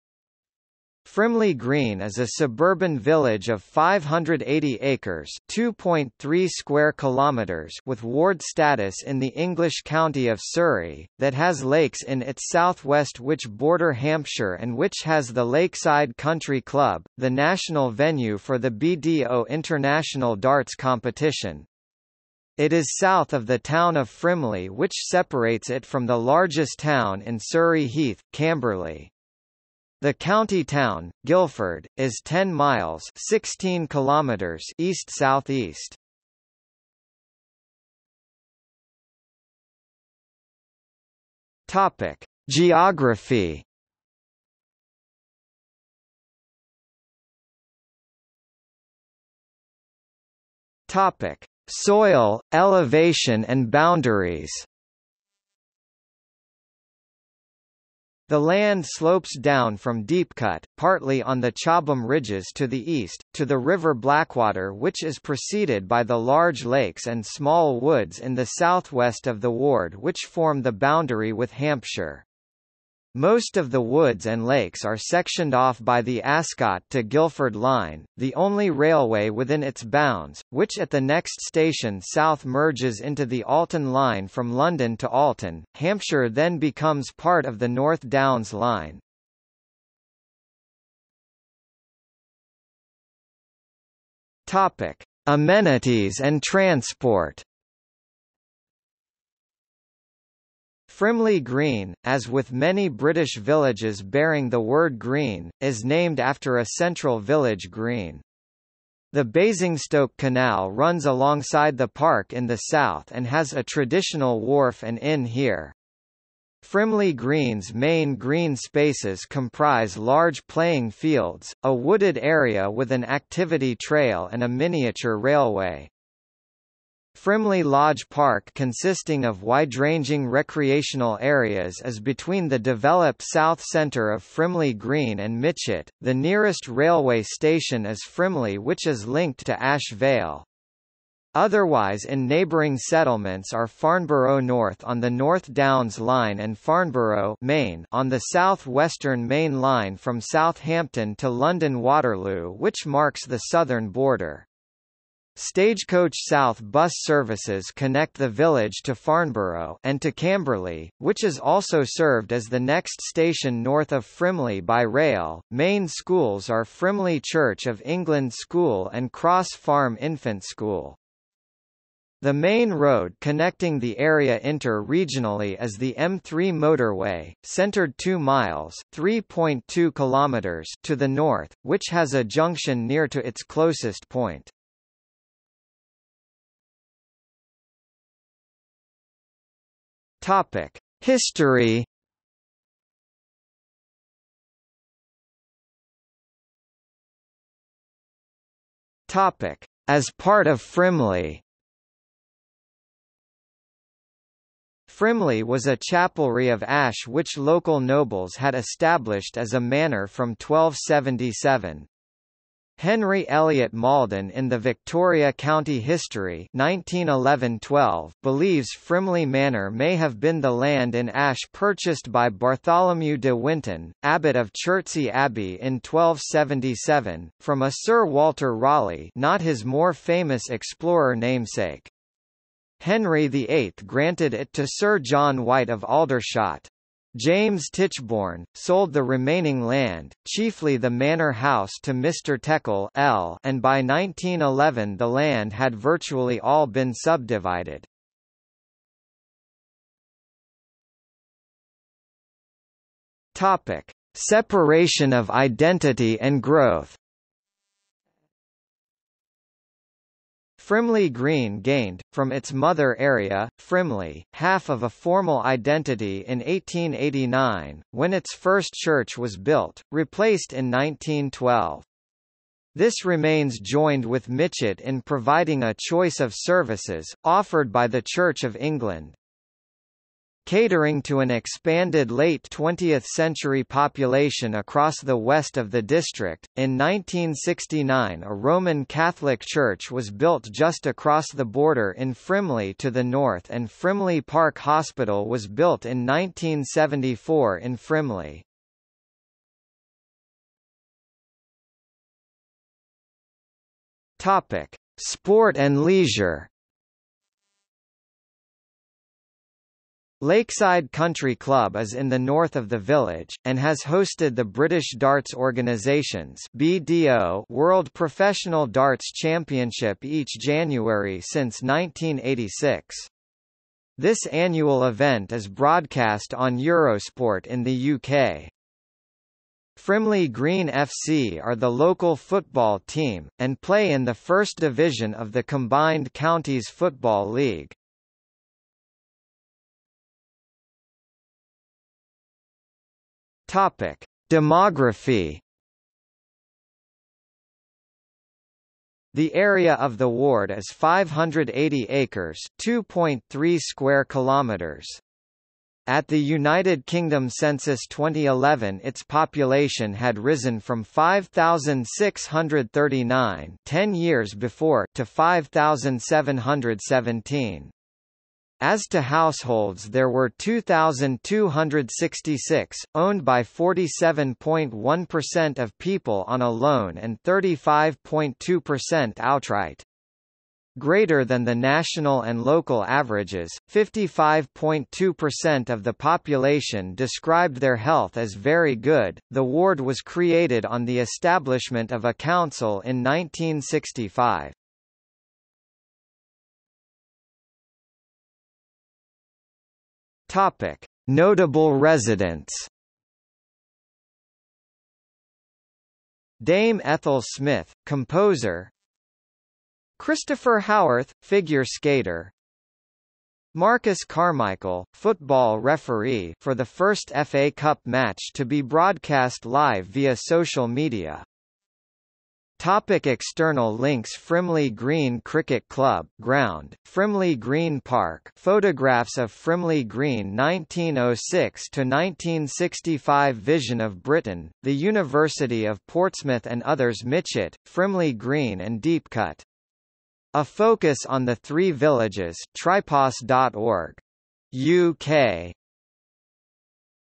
Frimley Green | Wikipedia audio article
00:00:38 1 Geography
00:00:47 1.1 Soil, elevation and boundaries
00:01:31 1.2 Amenities and transport
00:03:06 2 History
00:03:15 2.1 As part of Frimley
00:04:09 2.2 Separation of identity and growth
00:05:00 3 Sport and leisure
00:05:39 4 Demography
00:06:36 5 Notable residents
00:07:02 6 External links

- Socrates

SUMMARY
Frimley Green is a suburban village of 580 acres (2.3 km2) with ward status in the English county of Surrey, that has lakes in its south-west which border Hampshire and which has the Lakeside Country Club, the national venue for the BDO international darts competition. It is south of the town of Frimley which separates it from the largest town in Surrey Heath, Camberley. The county town, Guildford, is 10 miles (16 km) east south-east.